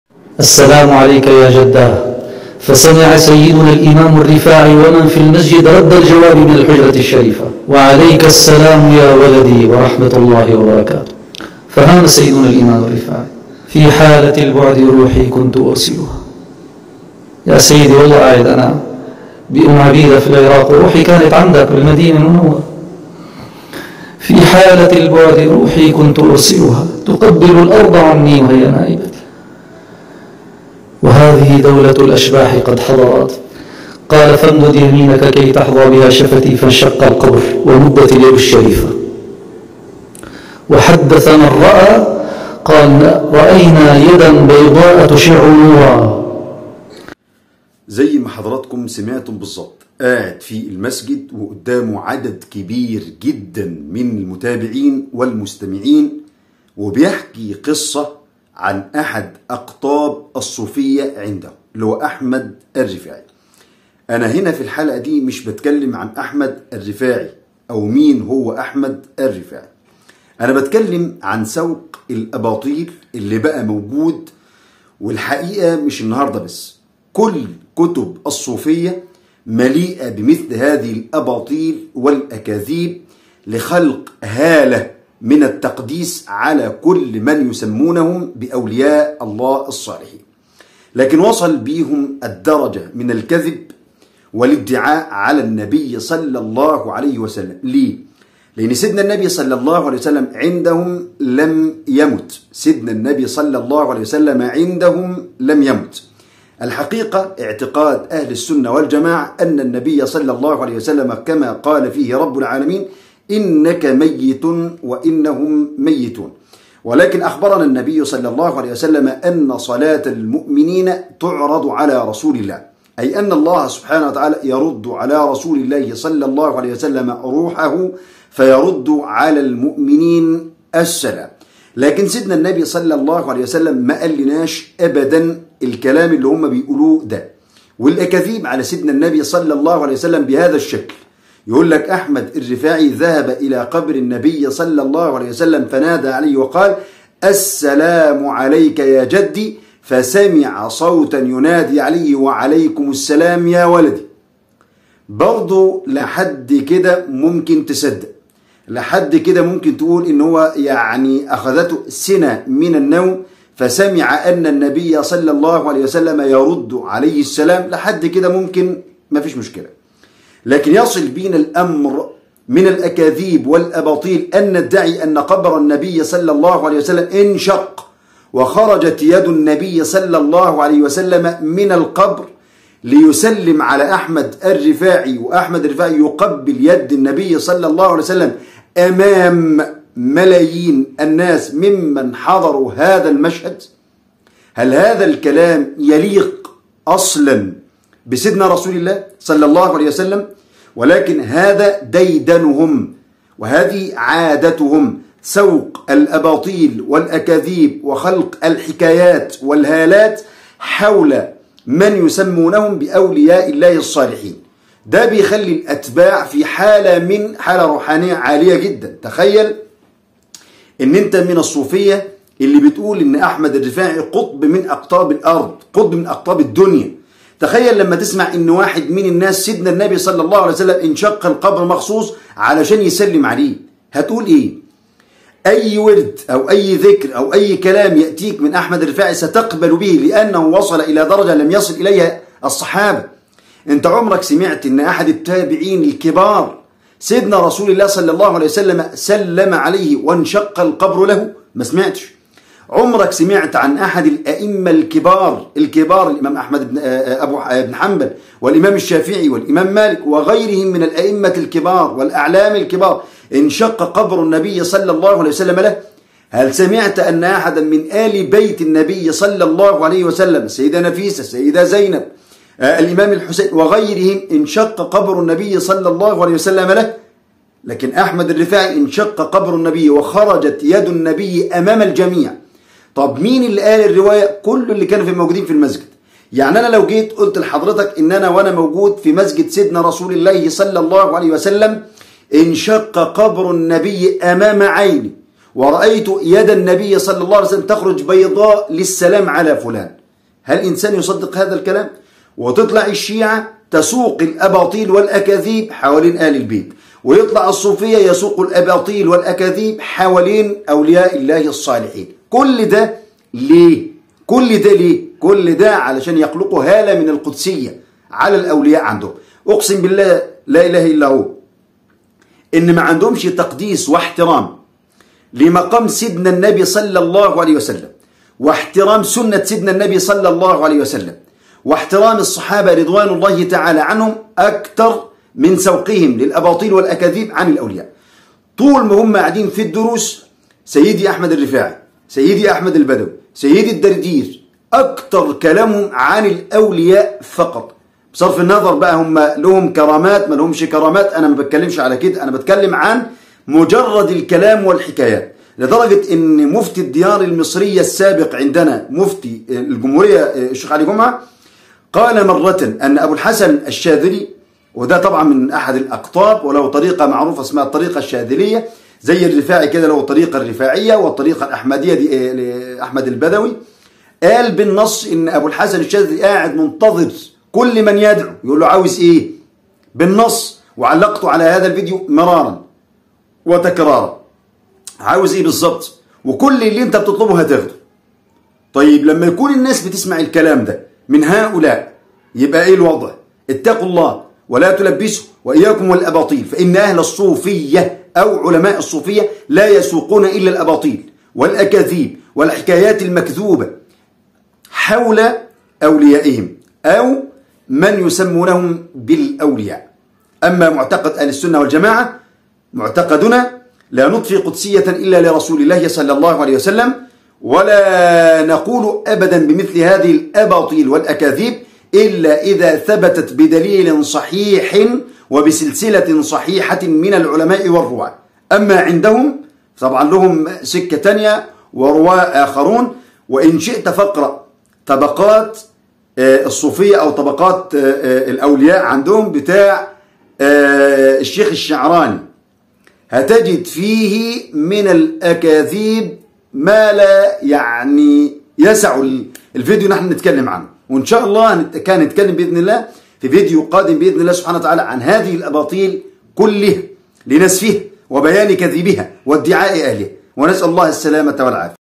الرفاعي : نادي علي النبي فرد عليه وانشق القبر وخرجت يدُ النبي وسلم عليه أمام الجميع فهل تصدق؟
أحد الأدعياء يقول بأن النبي صلى الله عليه وسلم انشق قبره  من أجل السلام على أحد أقطاب الصوفية